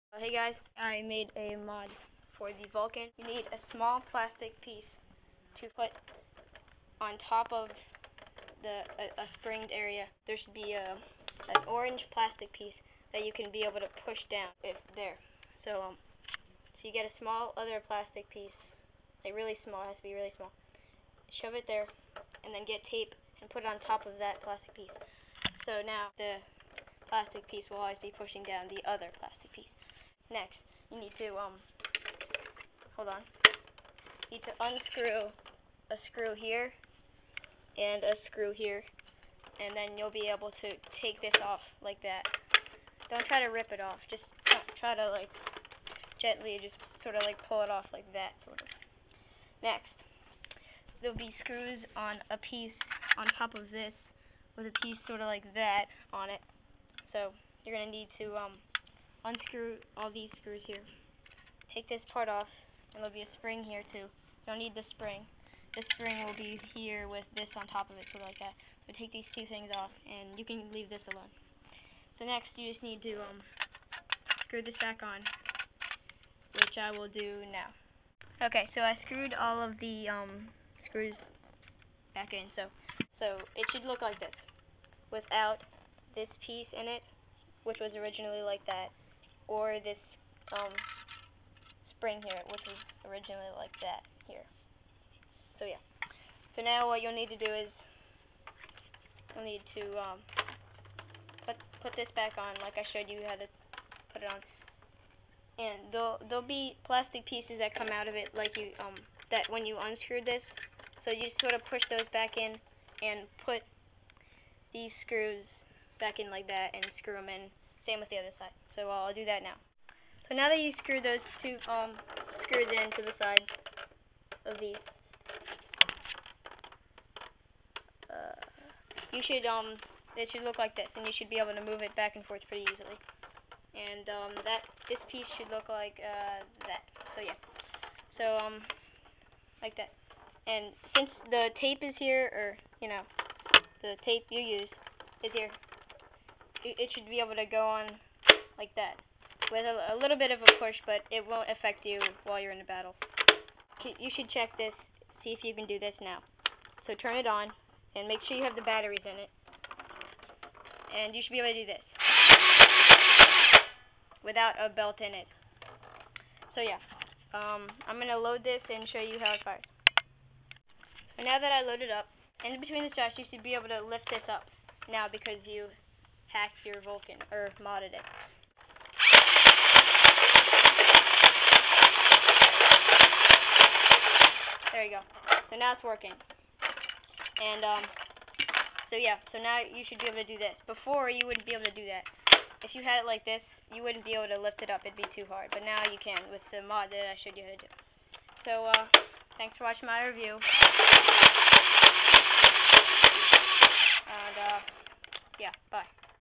Awesome Nerf Vulcan Mod
I demonstrate how to mod your Vulcan to make it able to be opened while firing.